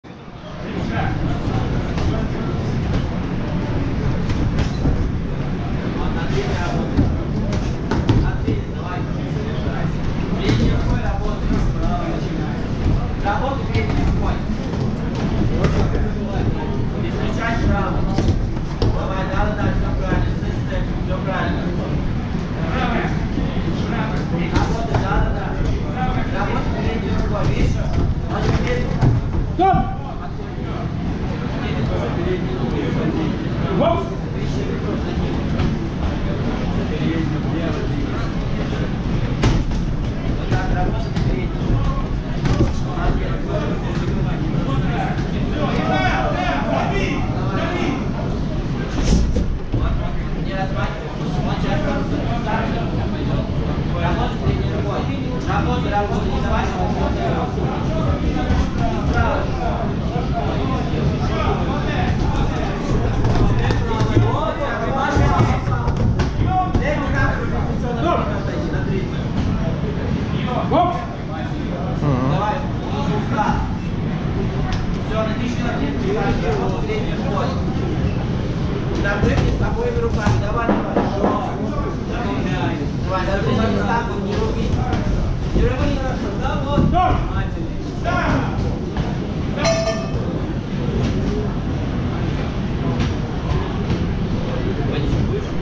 Щадиев Мохаммад Муратов Эльдар3
5 октября 2019 года. Турнир 7 легенд (1 турнир) - Кубок Бориса Лагутина. Зал на Кожуховской.